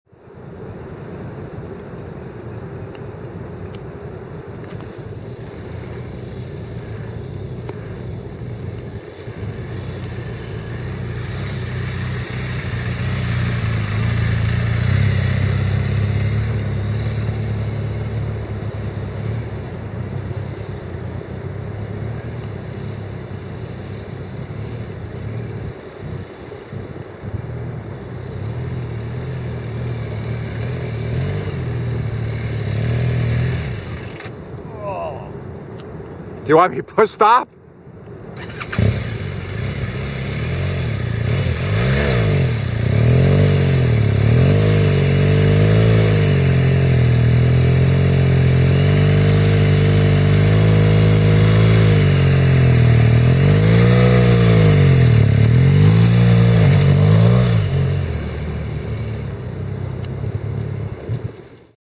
In the Sand
Driving a 600 pound motorcycle through the sand for the first time...  made it, not pretty, but made it.